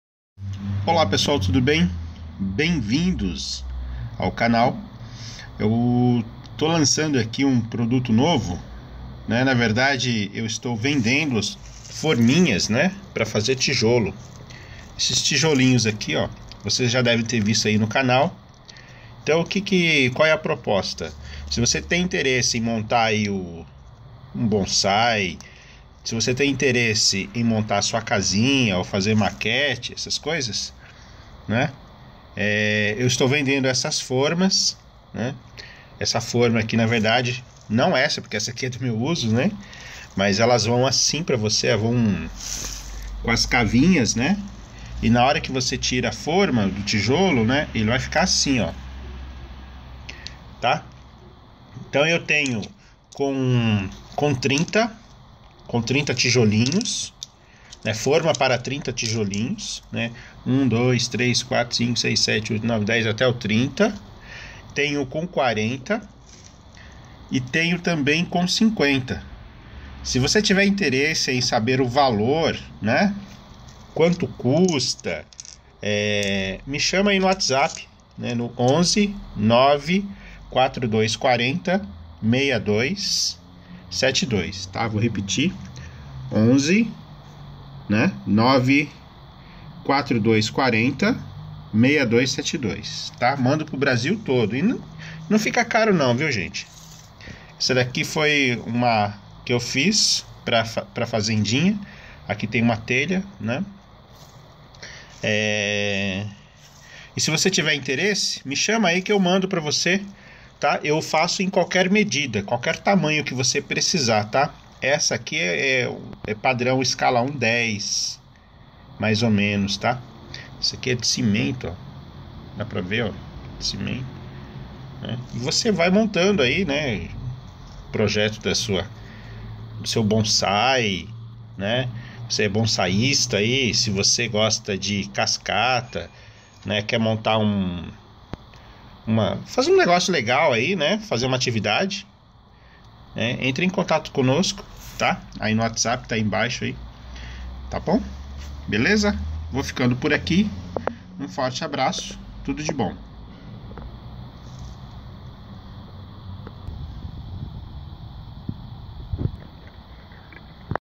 Como fazer Forma de tijolinho, Onde Comprar forma de Tijolinhos
Vendo Forma de Mini tijolinhos de Cimento, Como fazer Forma de Mini tijolinhos para Bonsai, Para Mini casinhas, Como fazer em casa!